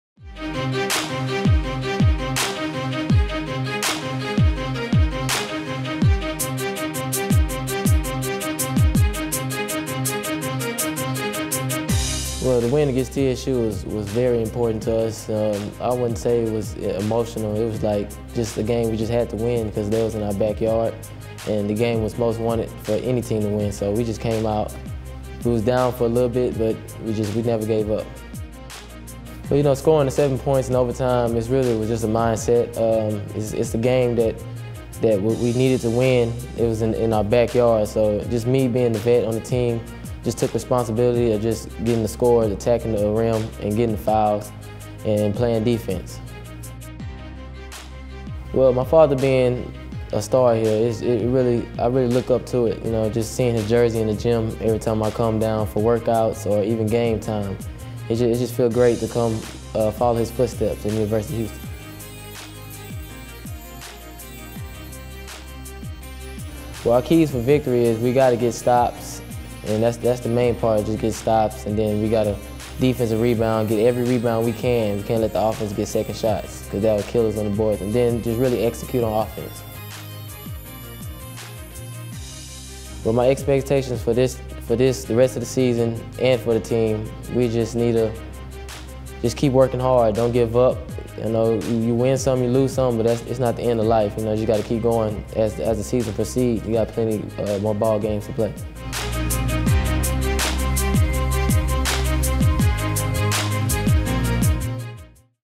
Joseph Young Player Spotlight
Check out this week's Men's Basketball Player Spotlight, Joseph Young, as he talks about last week's win over TSU and more!